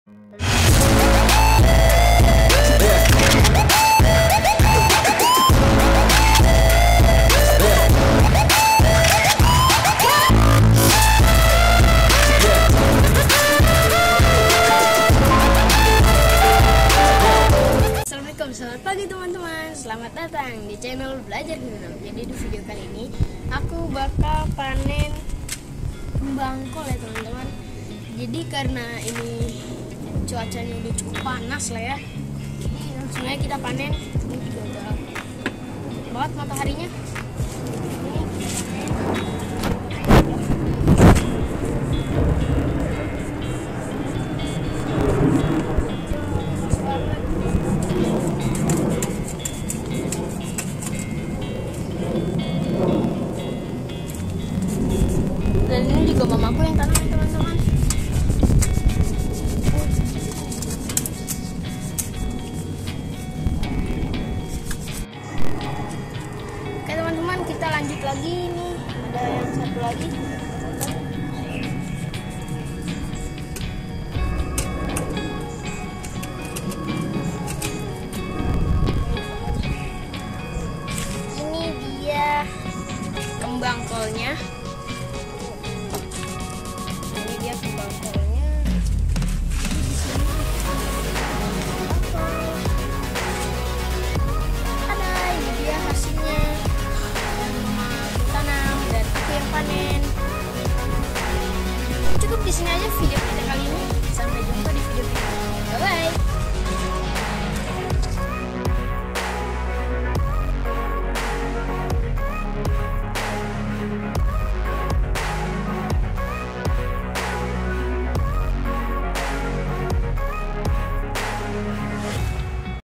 Panen kembang kol||kembang kol dalam pot|| mesti sabar nanam ini,karena panennya lama 😁
Assalamualaikum...
Haii..haii..para moms dan bunda,,apa kabarnya, semoga dlm keadaan sehat ya, kali ini Raffa mengajak moms dan bunda untuk panen kembang kol, menanam nya sangat mudah, hanya di dalam pot, dan memanfaatkan lahan yang ada, menanam di atap rumah, semoga video kali ini menjadi inspirasi bagi para moms dan bunda, oiya yg blm subscribe tolong di subscribe channel aku ya,,😁agar aku semangat 💪 lagi bikin video nya🙏 trimks

Salam hangat dari kami moms dan Raffa ❤️